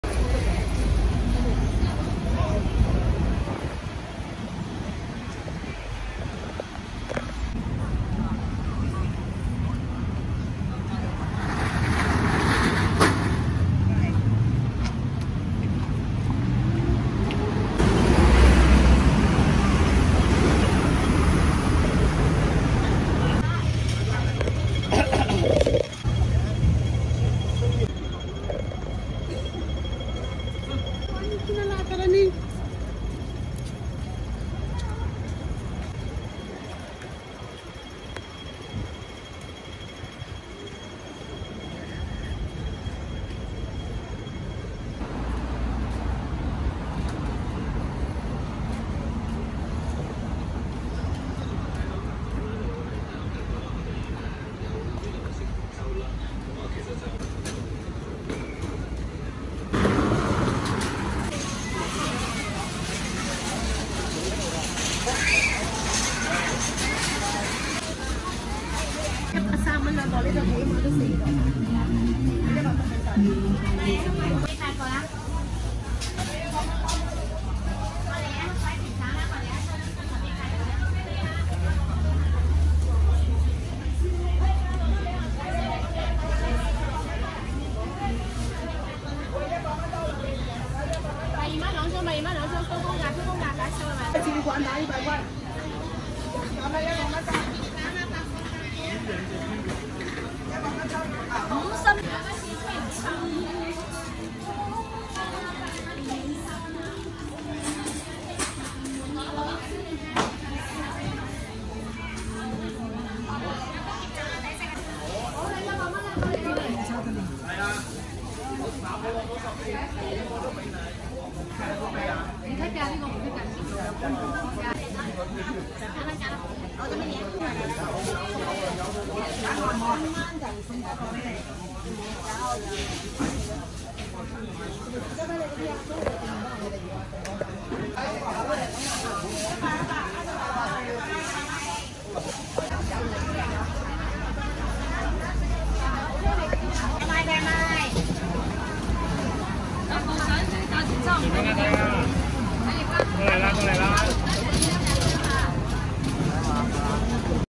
How To Go Fruits Wholesale Market Yaumatei-Hong Kong
While travel City Walk Tour in Hong Kong, you can have short & quick visit to Fruits Wholesale Market! Varieties of Fruits can find here!
Have a nice day & Enjoy!😊